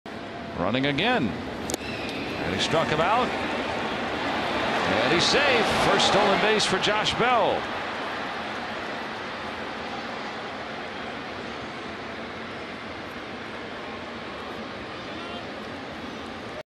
PIT@MIA: Bell swipes first career base
Josh Bell steals the first base of his career, swiping second base after Andrew McCutchen strikes out in the top of the 5th inning